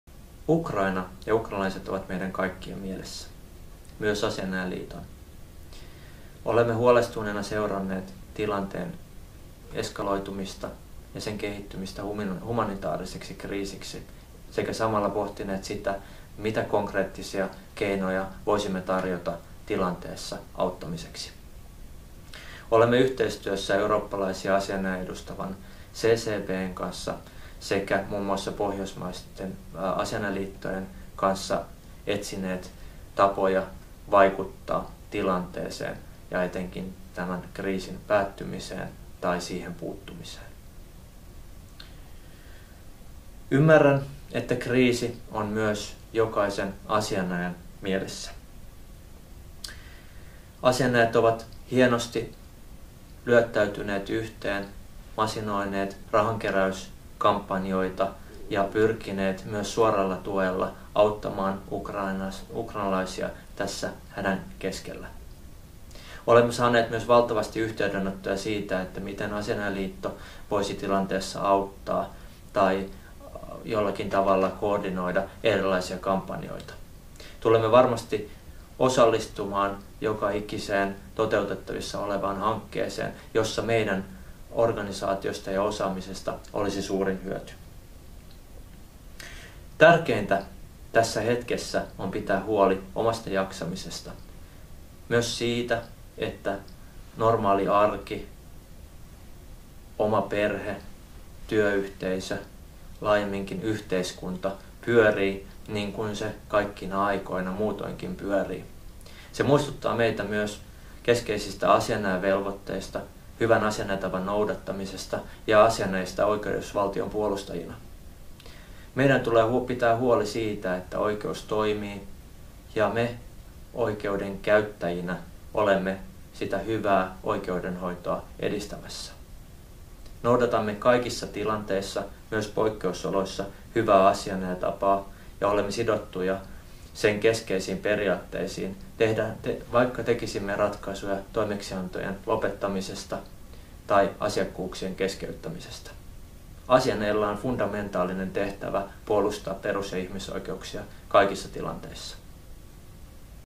Pääsihteeri Niko Jakobssonin Ukrainan tilanteeseen liittyvä viesti asianajajille
Asianajajaliiton pääsihteerin Niko Jakobssonin ajankohtaiskatsaus jäsenille liittyen Ukrainan tilanteeseen.